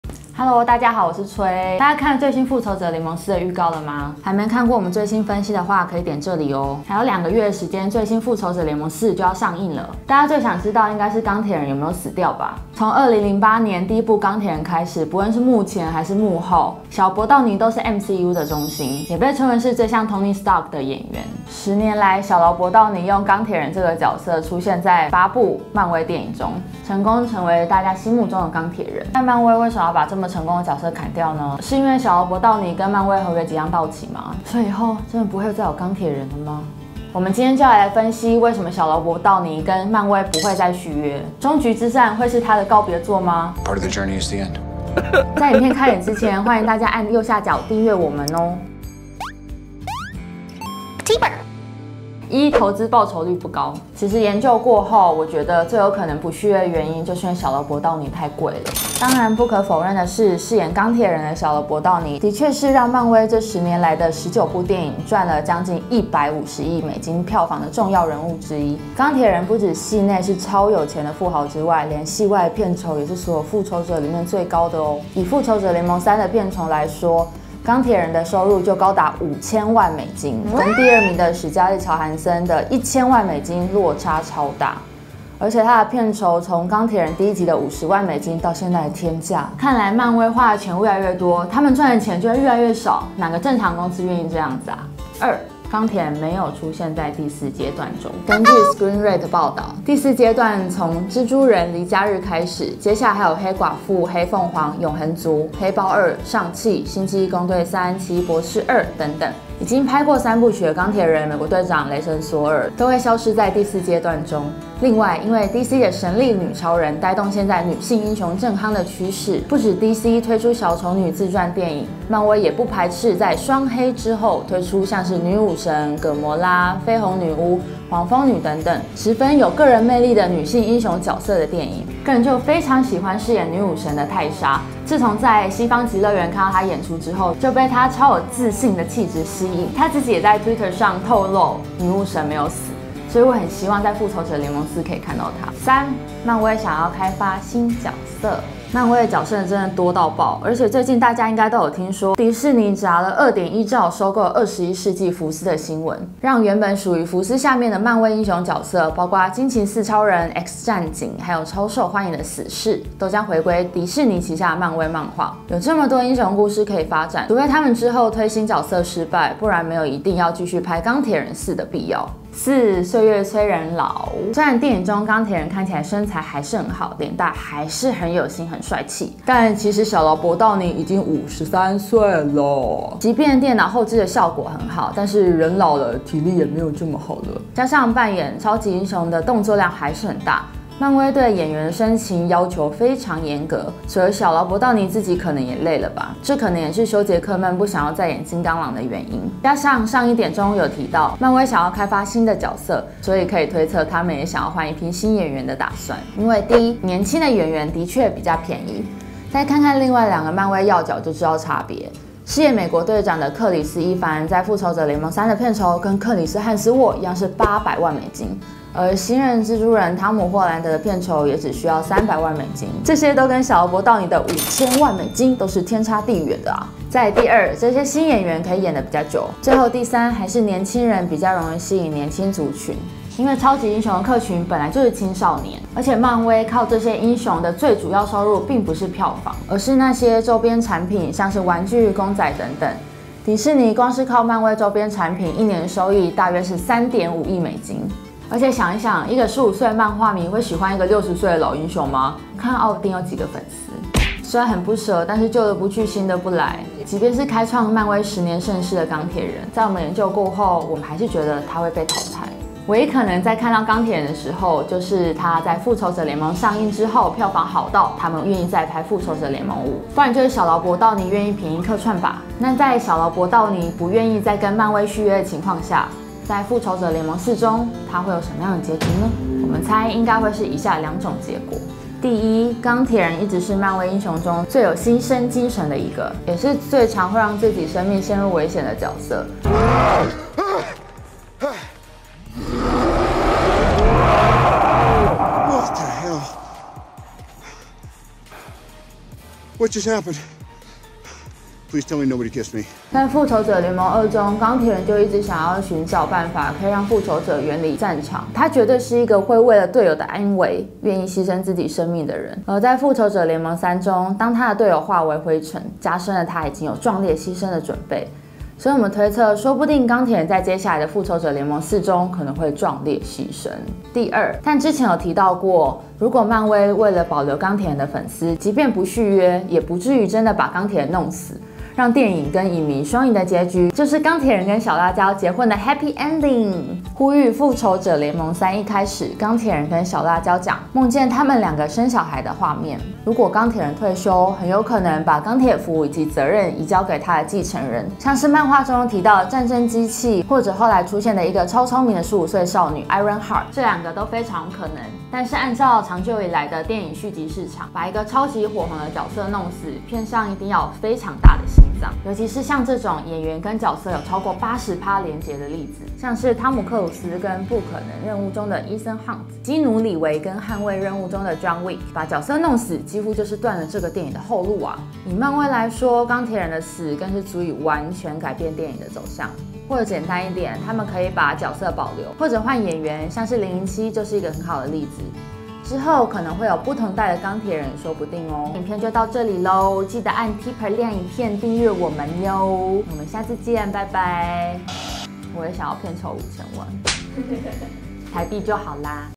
復仇者聯盟4結局鋼鐵人會死？4個漫威電影中不再出現鋼鐵人的理由｜電影情報
從2008年的第一部《鋼鐵人》開始，不論是在幕前還是幕後，小勞勃道尼都是MCU的中心，也被稱為是最像Tony Stark的演員。10年來，小勞勃道尼用鋼鐵人這個角色，出現在8部漫威電影中，成功的成了大家心目中的鋼鐵人。但為什麼漫威要把這麼成功的角色砍掉呢？是因為小勞勃道尼跟漫威的合約將要到期，所以以後不會再有鋼鐵人了嗎？我們今天就要來分析為什麼小勞勃道尼跟漫威不會再續約，終局之戰會是他的最後告別作嗎？